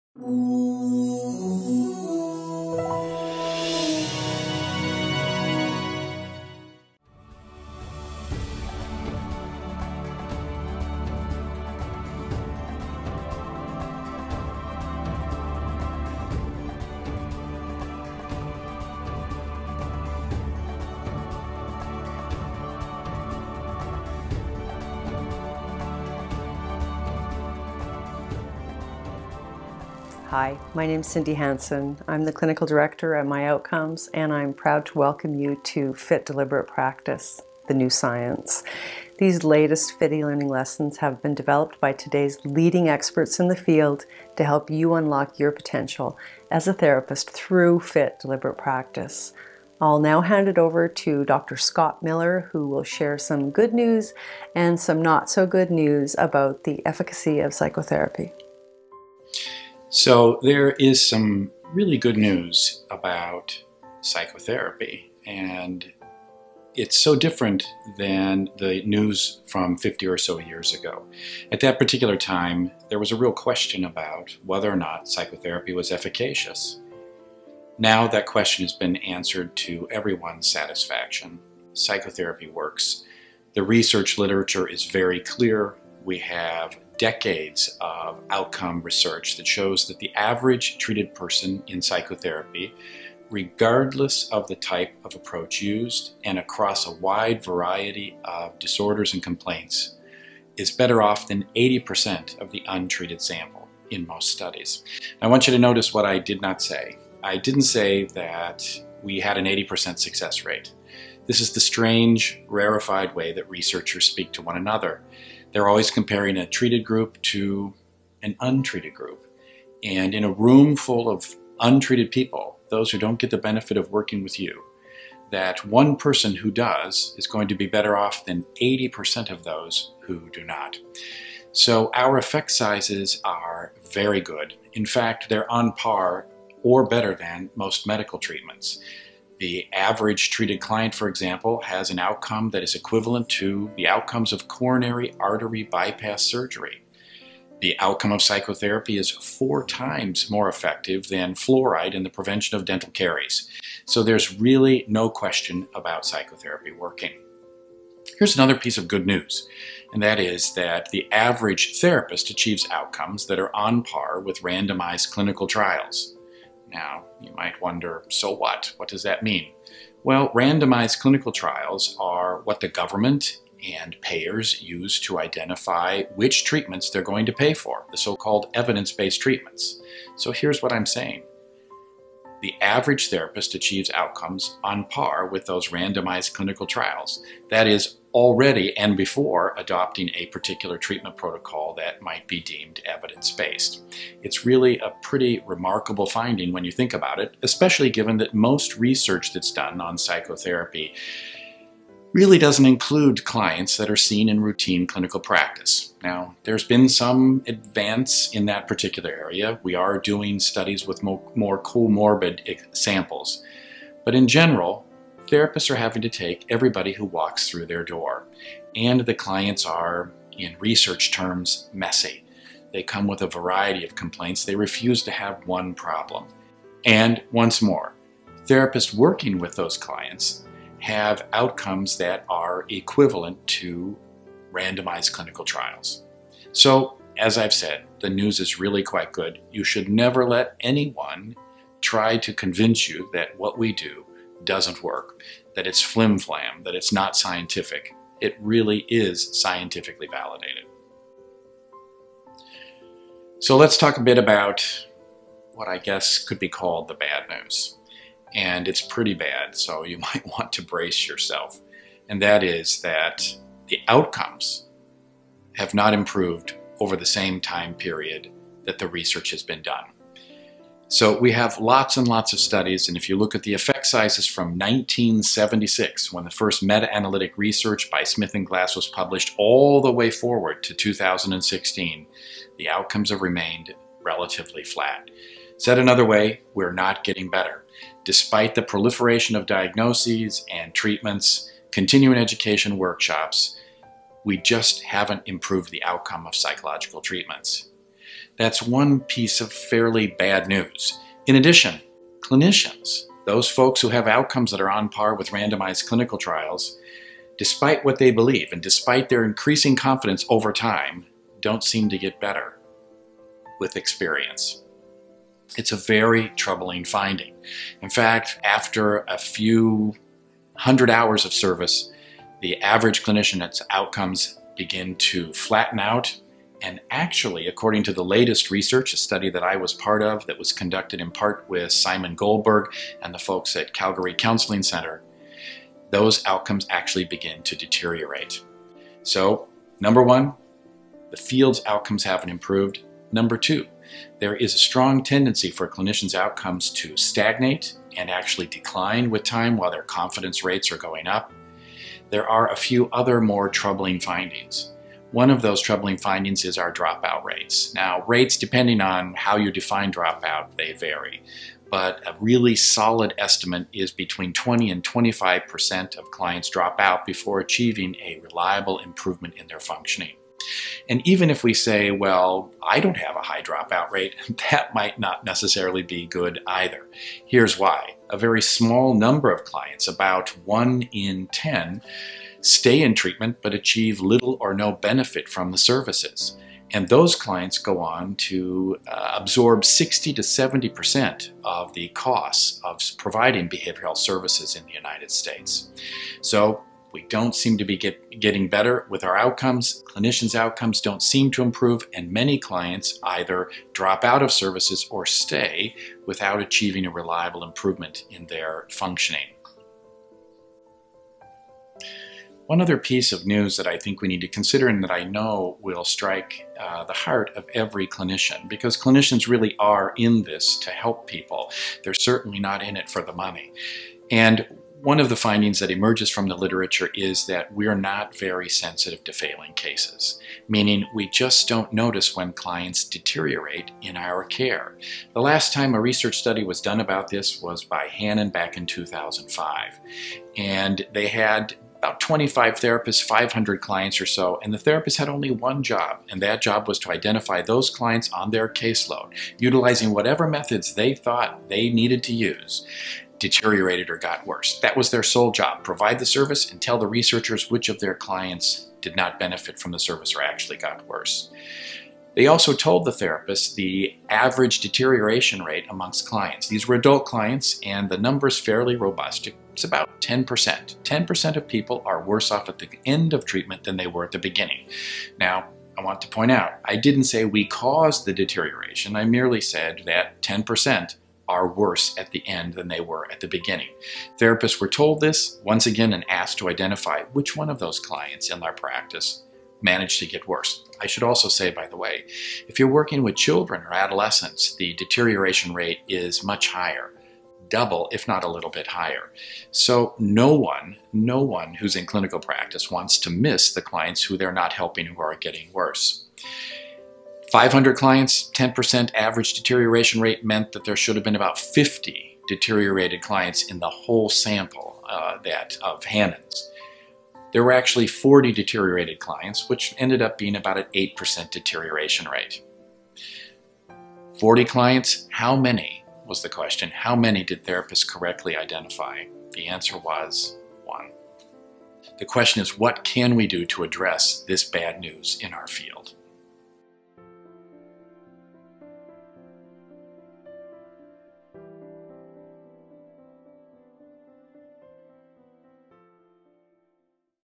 Scott Miller PhD, introduces FIT Deliberate Practice
FIT Deliberate Practice…the New Science

If you find yourself wondering, “What could I have done differently to keep them engaged?” or “Why didn’t they rebook?” then you should consider FIT Deliberate Practice

It’s been only four years since preliminary results on the new science of FIT Deliberate Practice were first published by Scott D. Miller, Daryl Chow, Sharon Lu and Geoffrey Ta. Turns out that the factor that separates the best from the rest is the amount of time spent practicing, but not just any kind of practice, deliberate practice. A conscious and continuous effort aimed at making incremental and measurable improvements to your performance.

FIT Deliberate Practice:
-Applies the science of expertise to the field of mental health.
-Contains cutting edge information and the production values are simply fantastic.

Provides a roadmap for addressing a fundamental issue in mental health: How can therapists improve and become experts?

Will help you develop an individualized plan for improving your effectiveness based on the latest scientific evidence on expert performance.

As you work through this course there will be a number of demonstrations, exercises and quizzes designed to help you establish a baseline personal profile to work from in developing your FIT Deliberate Practice goals.